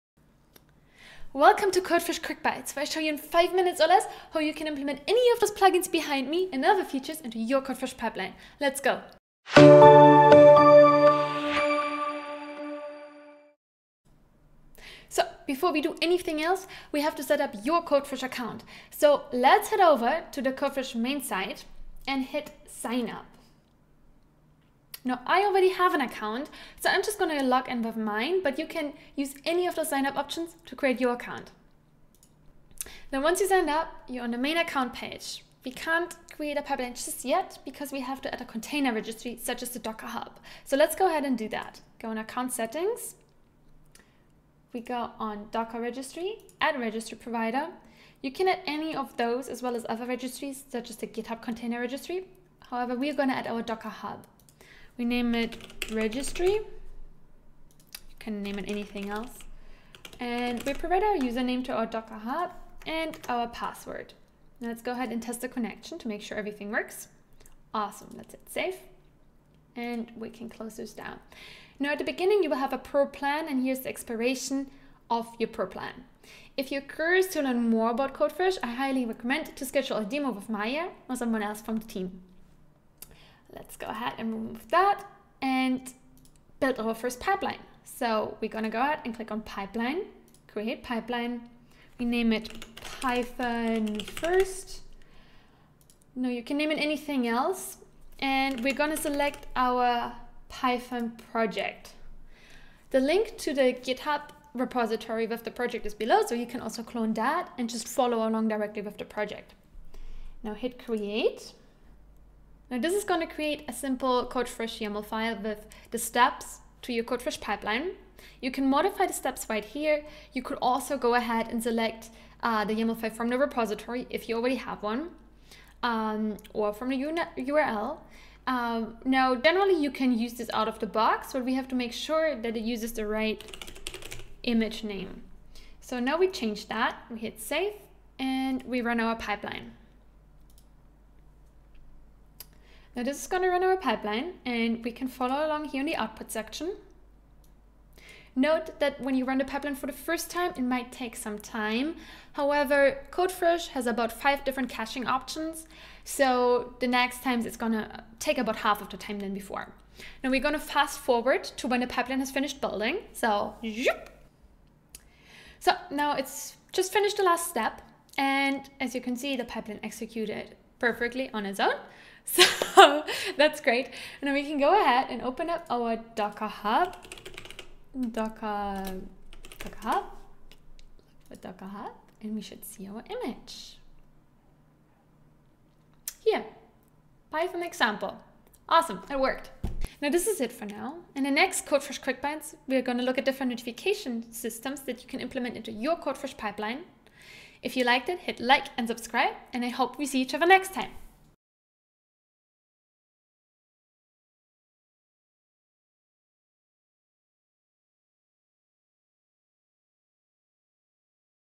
Codefresh Quick Bites: Set-up your account
This is the first part of a new series -- Introducing Codefresh Quick Bites -- Set-up your Codefresh account and run your first pipeline!